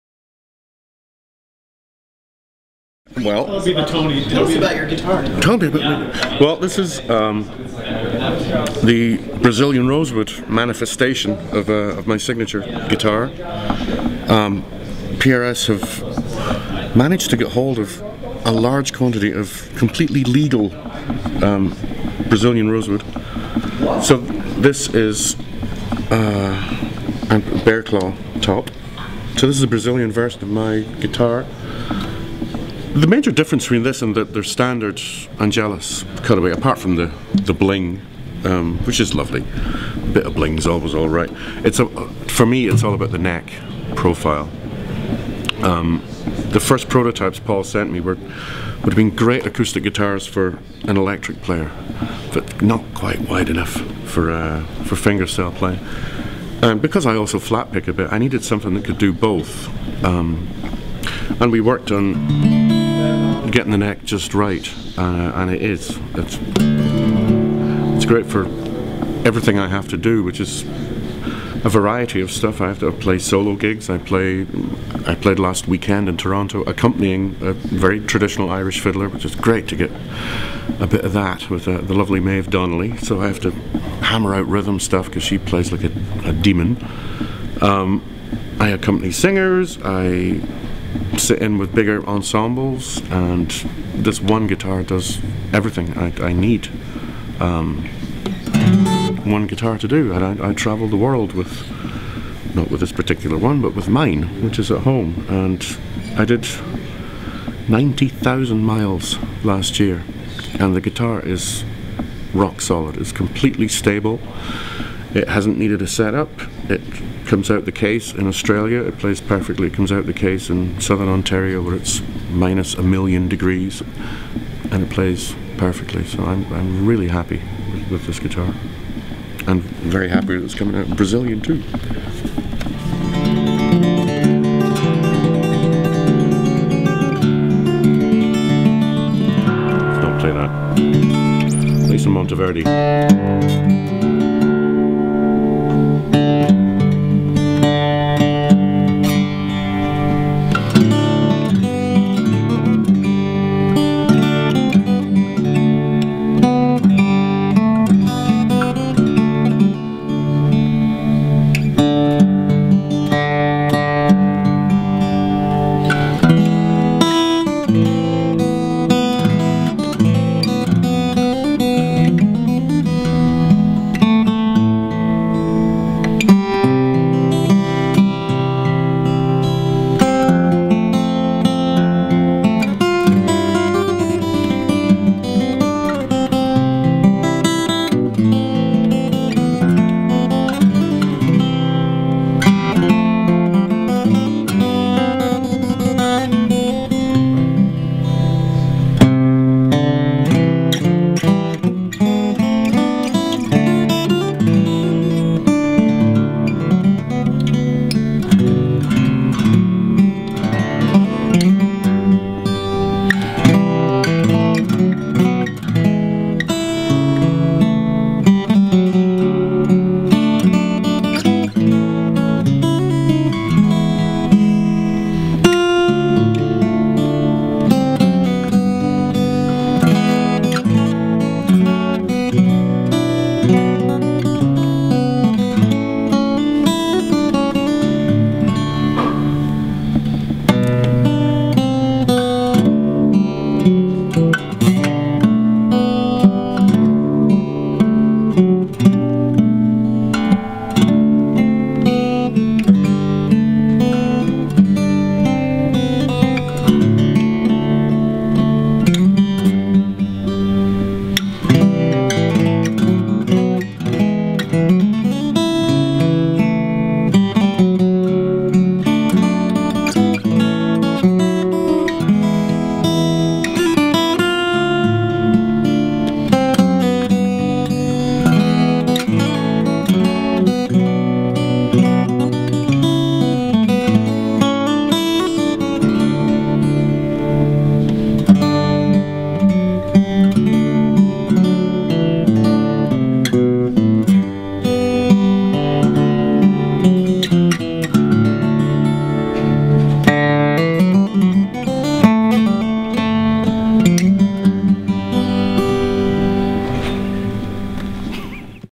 Tony McManus and PRS Guitars at Winter NAMM 2013 from Acoustic Guitar
Guitarist Tony McManus discusses and demonstrates the new Brazilian rosewood version of his Paul Reed Smith signature model at Winter NAMM 2013. For more coverage of acoustic guitar and gear, please